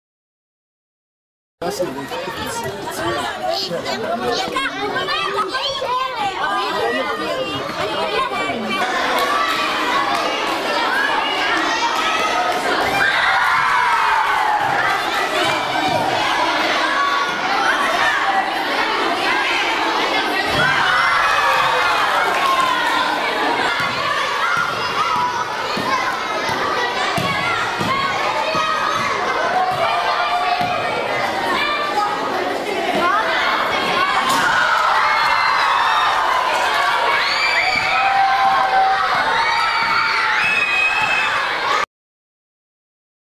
Oz Blayzer and Amir Celestin with Highlights at Ben Gurion School
עוז בלייזר ואמיר סלסטין עם היילייטס בבית ספר בן גוריון שברוממה, כחלק מהפעילות הקהילתית של הקבוצה